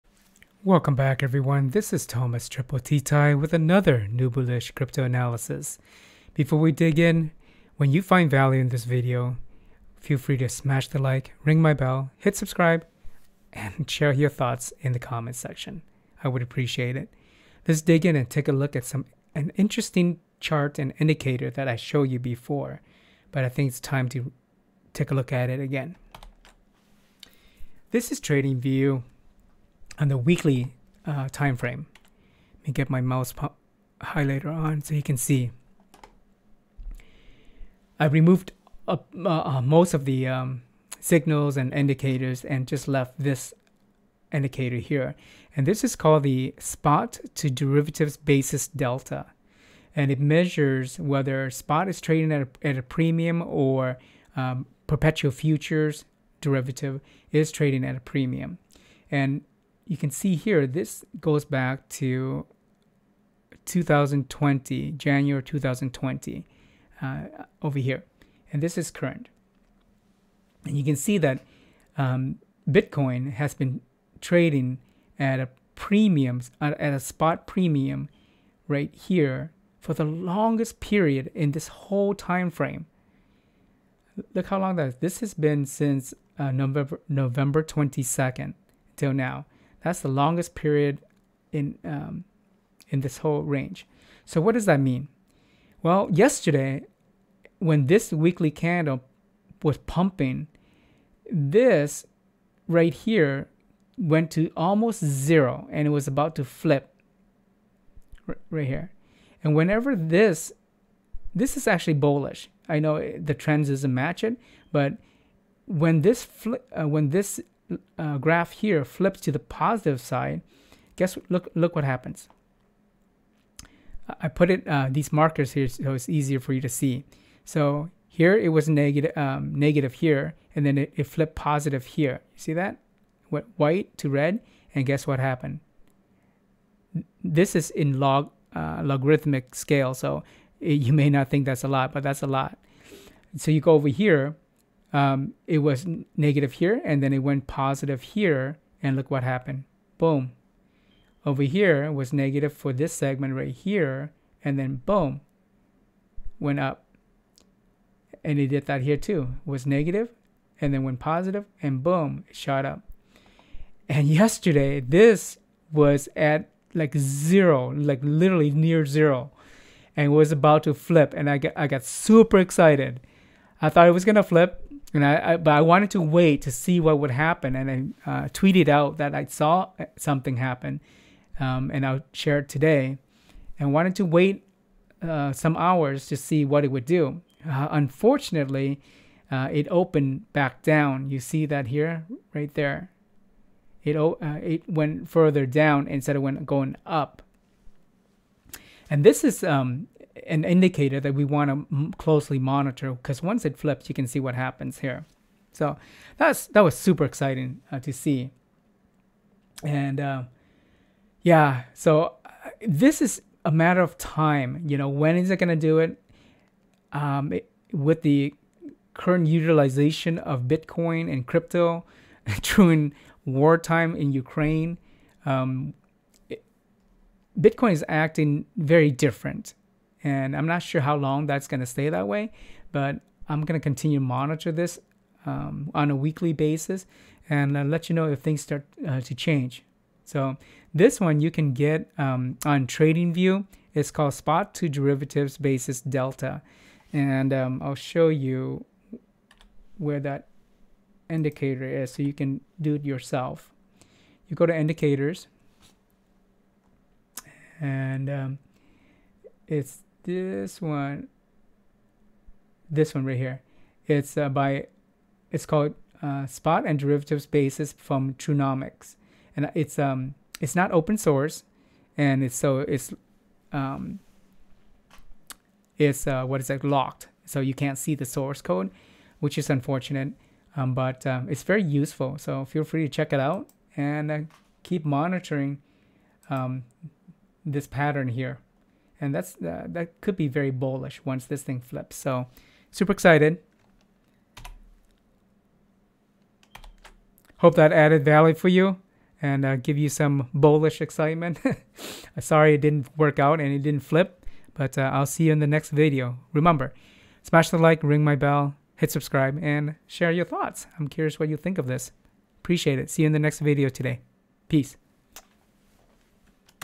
Bitcoin's Extremely Bullish Signal: Spot to Derivatives Delta Almost Flipped Positive!
Note: YouTube may still be processing higher resolution versions of this video.

BTC has been trading with the spot market at a premium since Nov 2021. The longest period in the years the data goes back to. It looked like it was going to flip yesterday from the BTC move that could signal a return to a bull market.

Find out what happened!